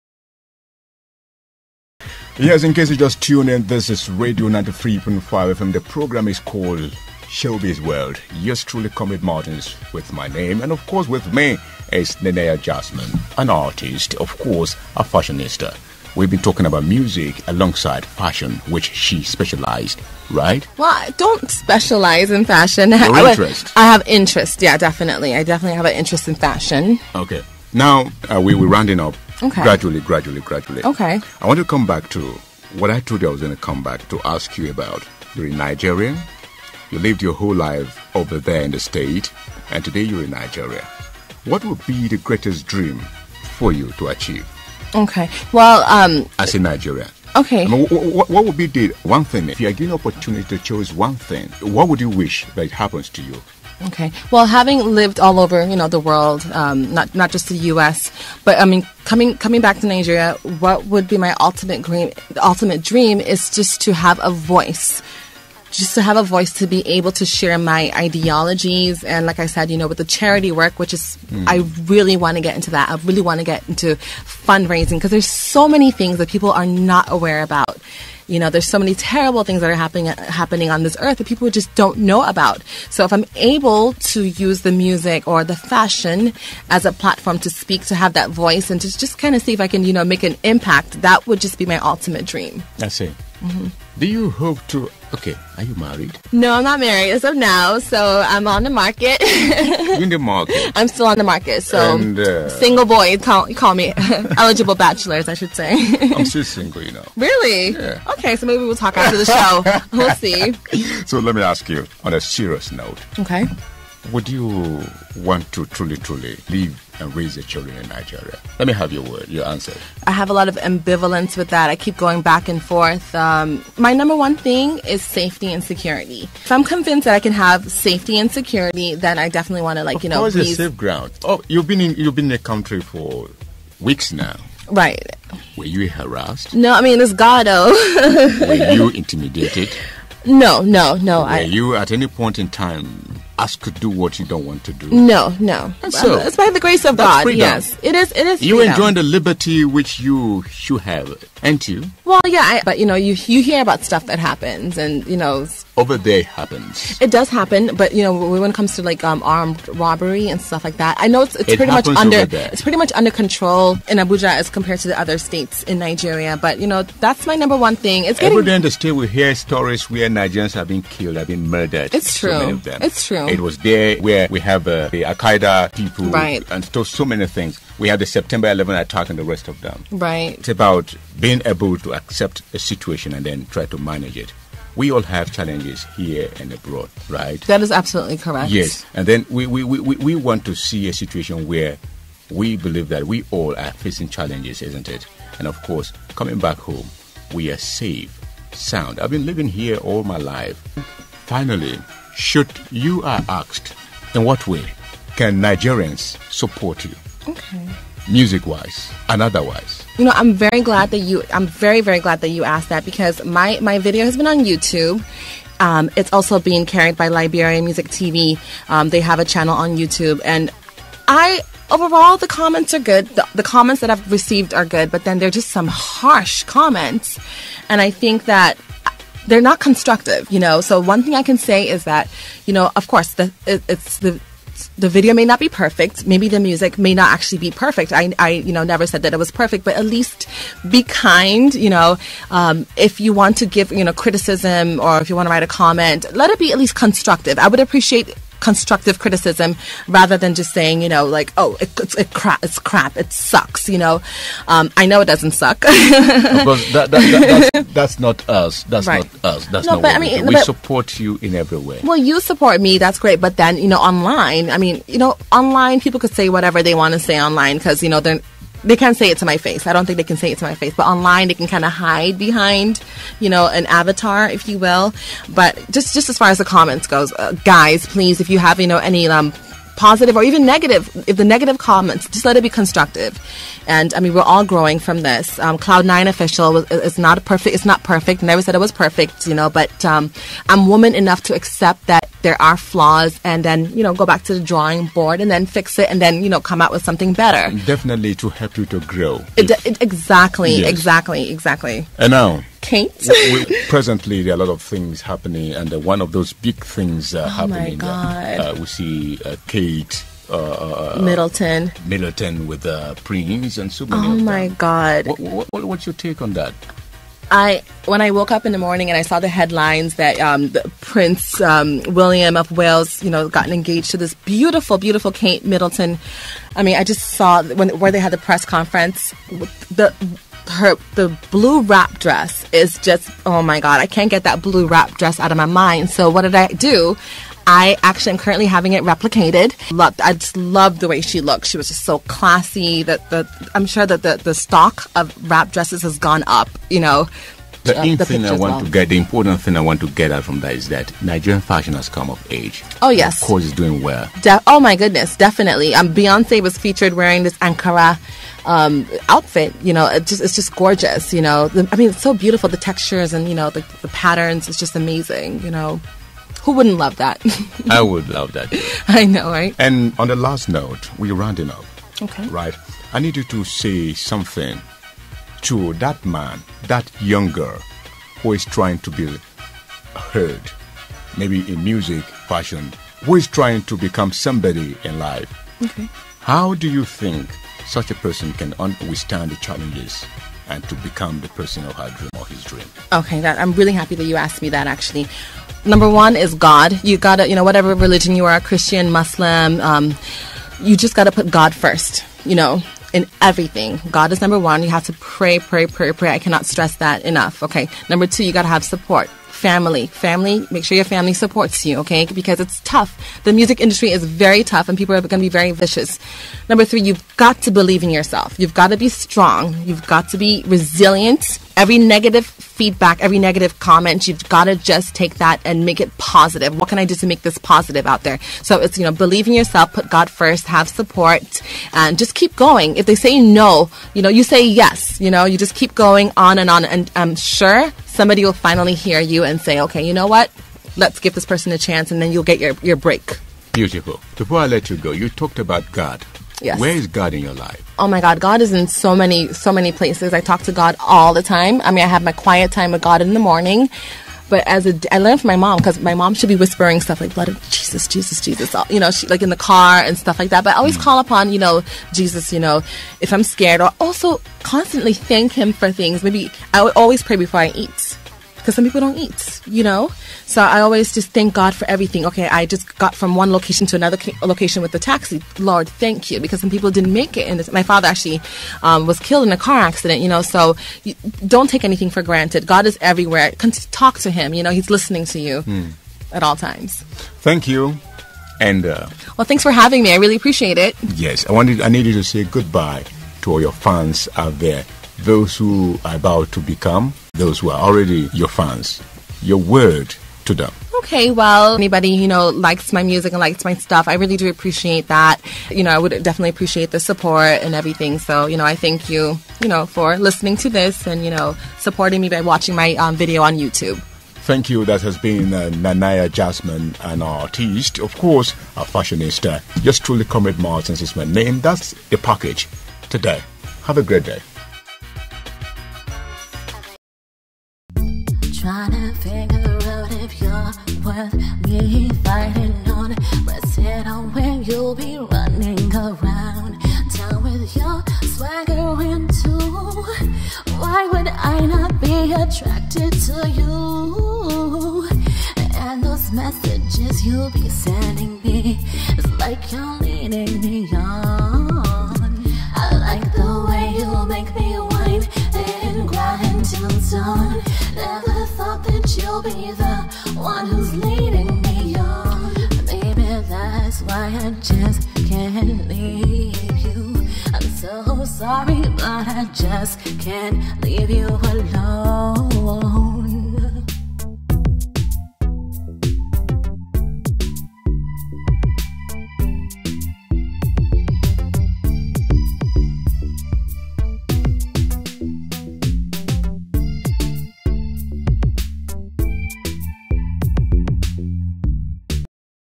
Nenaya Jazmine talks about Nigerian fashion, her music and Kate Middleton's dress.
Interviewed by Comrad Martins of Aso Rado, Abuja Nigeria.